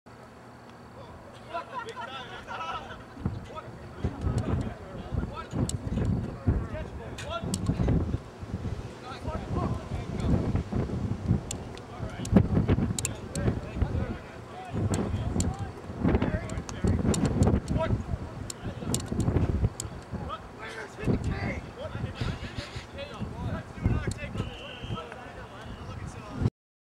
Berkman's Bank: Bucket Drill
Video footage of Salisbury men's lacrosse coach Jim Berkman's bucket drill. Finishers will love it.